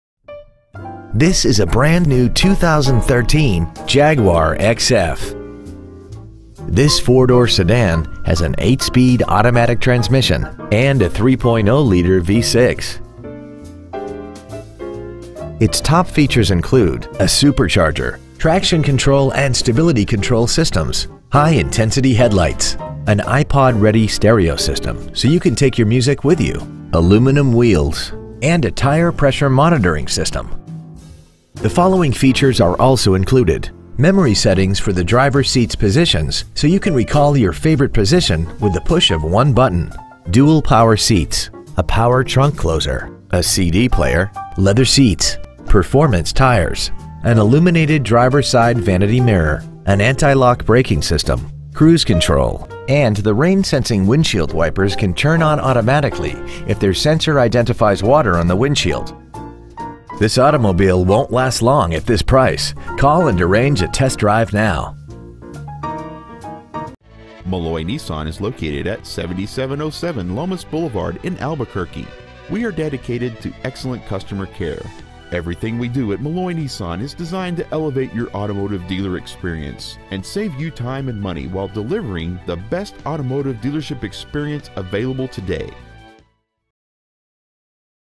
2013 Jaguar XF Albuquerque NM 87110
Year : 2013
 Make : Jaguar
 Model : XF
 Engine : Supercharged Gas V6 3.0L/183
 Trans . : Automatic
 Exterior : Italian Racing Red

 Stock : J1479

 7707 Lomas Blvd.

 2013 Jaguar XF Albuquerque NM